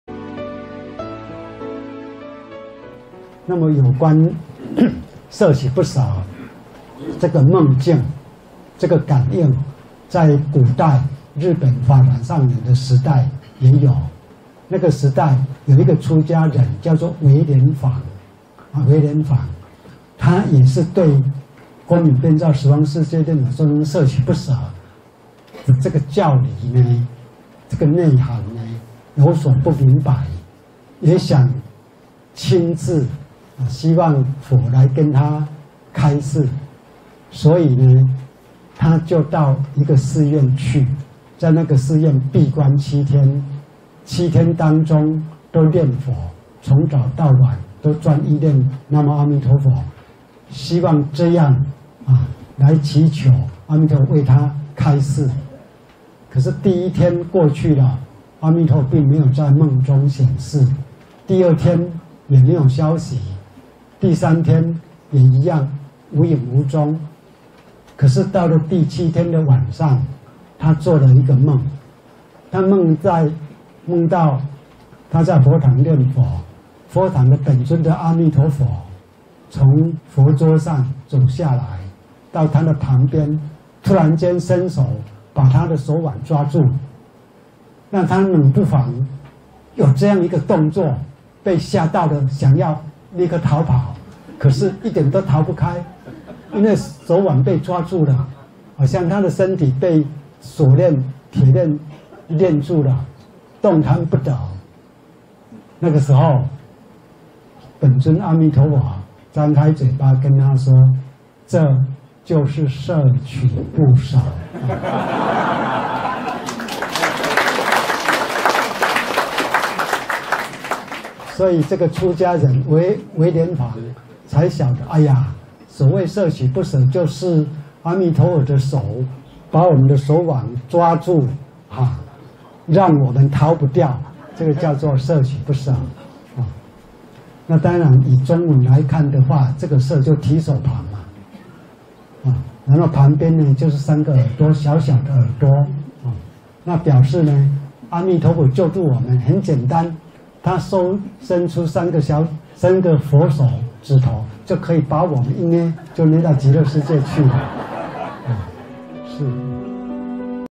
(염불감응) 섭취불사에 대한 감응사례 - 혜정법사 법문
미타구도 섭취불사(彌陀救度 攝取不捨)에 대한 감응사례

- 일본 유연방(唯蓮坊, Yuiren-bō) 스님의 일화
- 혜정慧淨법사 법문, 시견스님 번역 및 자막(200306)